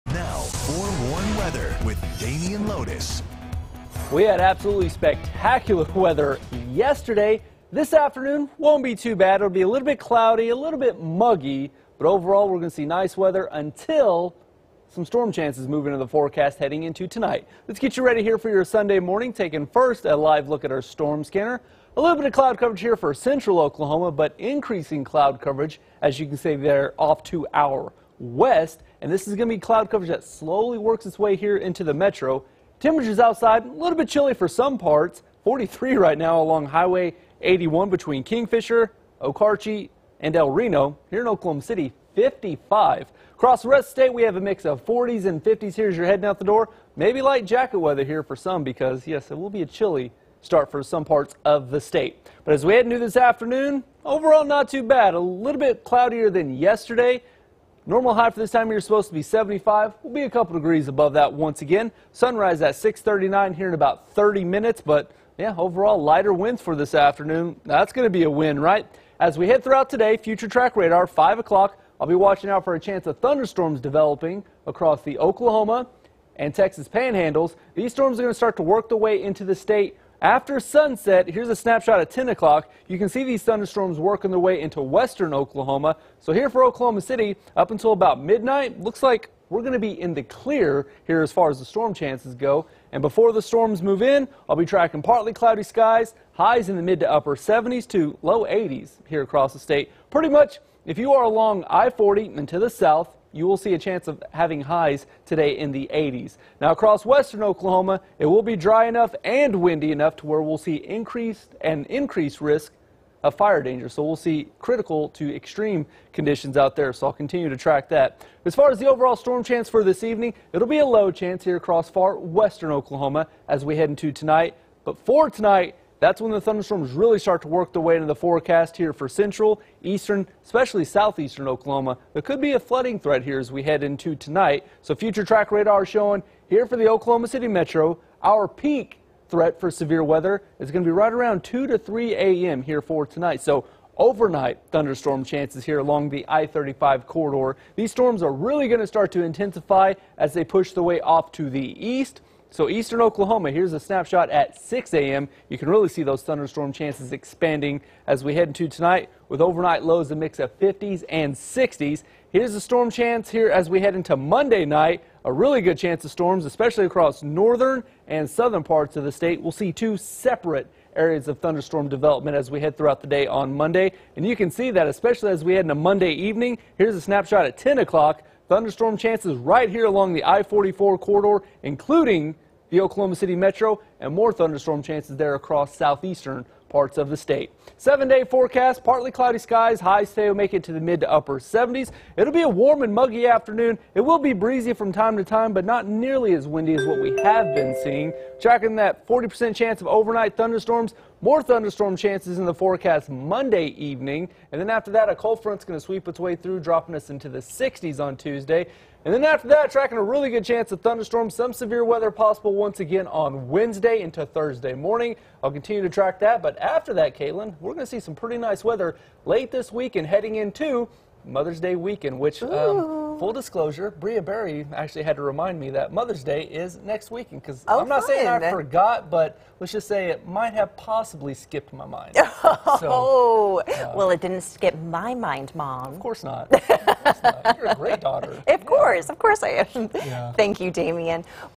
May 1st, 2022 Forecast
Storms move in tonight.